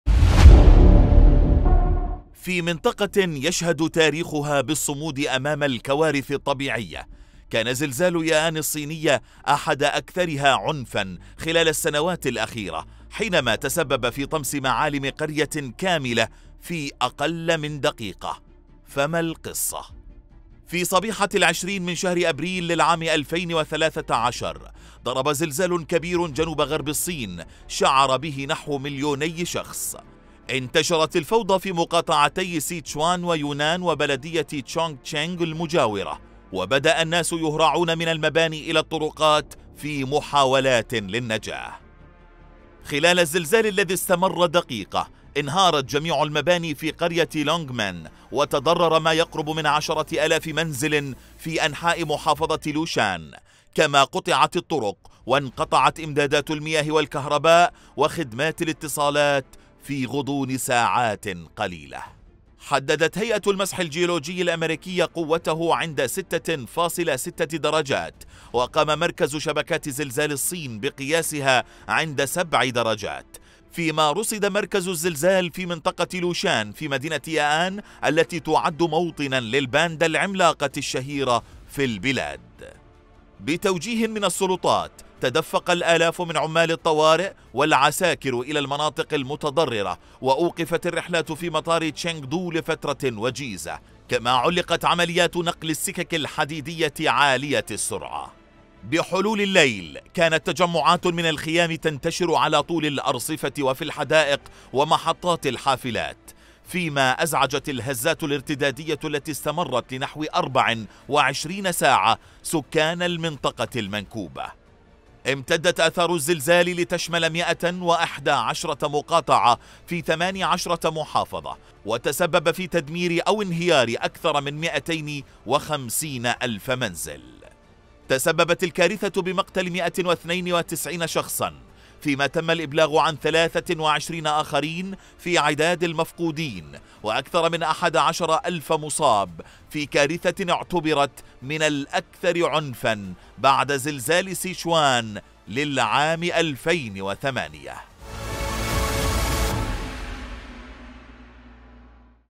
زلزال يآن الصينية نكبة أبريل
بسبب هذا الزلزال تشرد سكان قرية كاملة انهارت مبانيها، وتأثر نحو مليوني شخص في جنوب الصين.